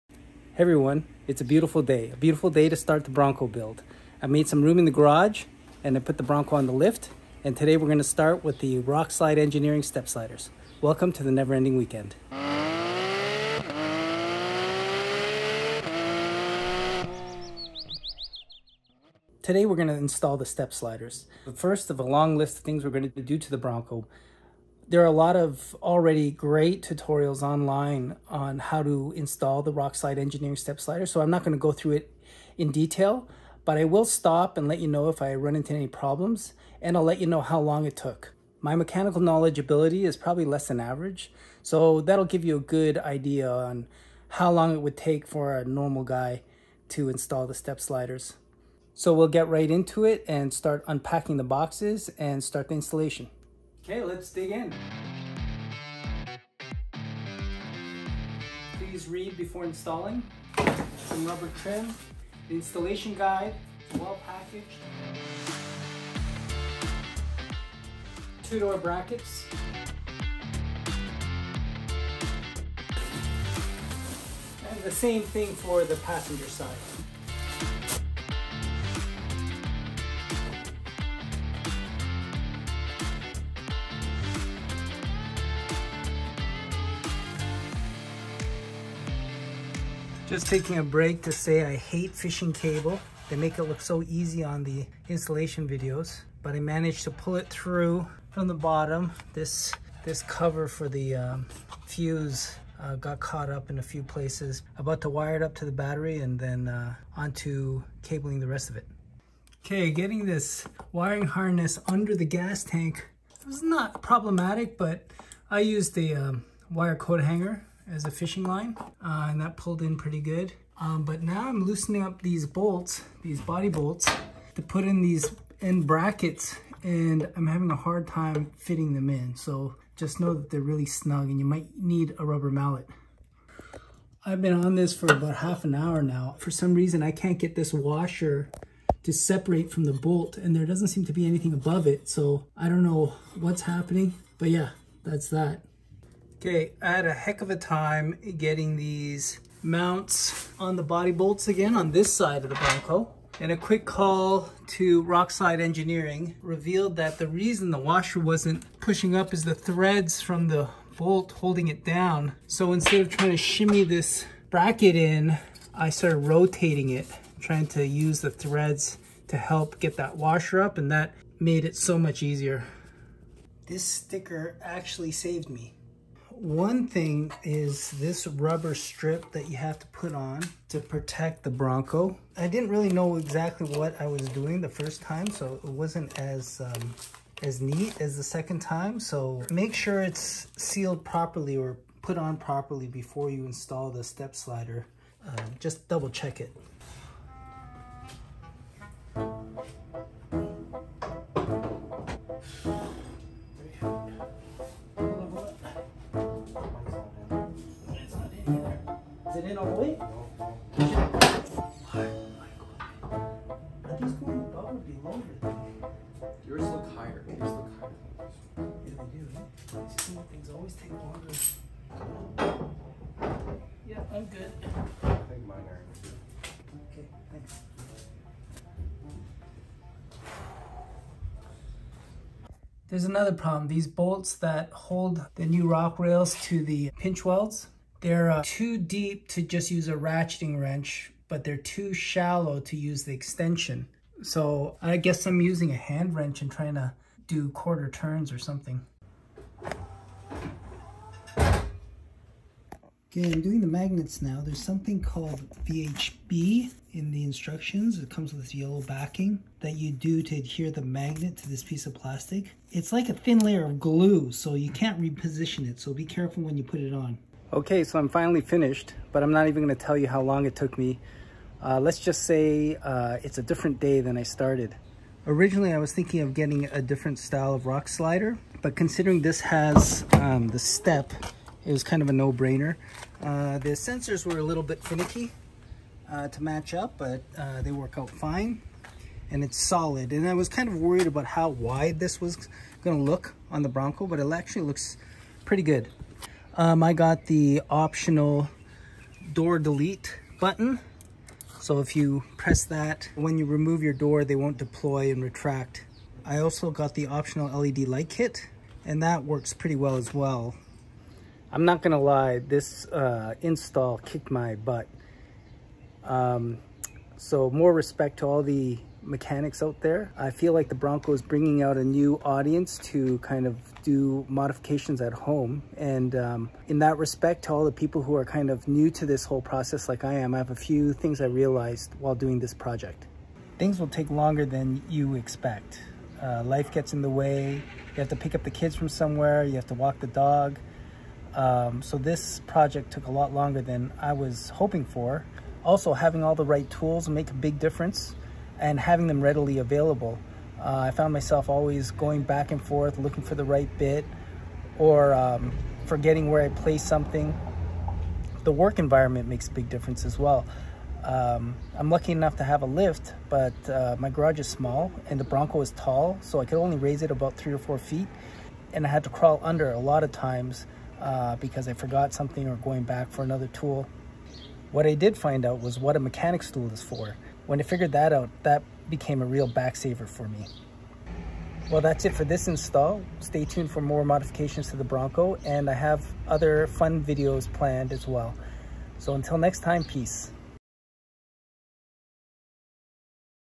The Biggest Improvement For Living With Sasquatch? Rock Slide Engineering Step Sliders - Build Day 1
Today we install the first mod to the Bronco: Rock Slide Engineering Step Sliders. Not really an installation tutorial, but I do stop along the way when I get stuck on something that isn't fully explained in the instructions.

The instructions from Rock Slide Engineering are pretty comprehensive but they skim over a lot of the final steps. The 2 best video tutorials I've found on YouTube are very comprehensive.

Let me know if you have questions or any suggestions on what I should install next.